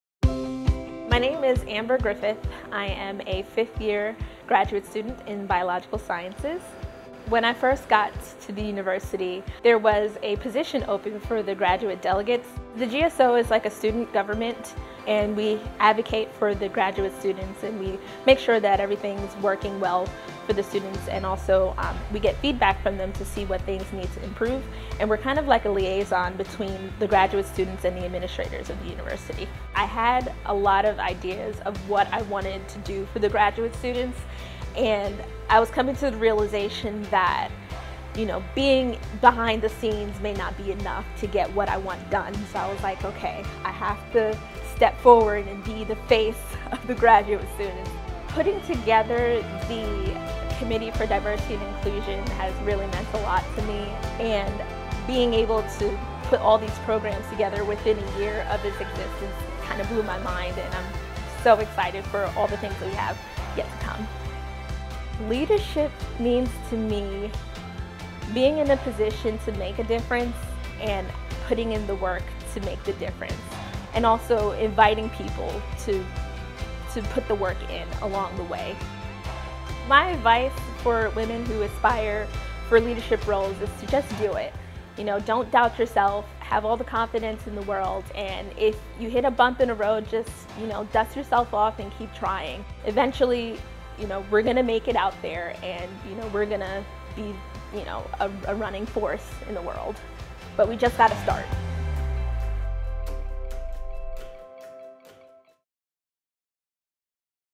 Spotlight on Women Leaders: Amber Griffith
Amber Griffith is pursuing her PhD at Pitt, but she still finds time to advocate for the graduate students of the University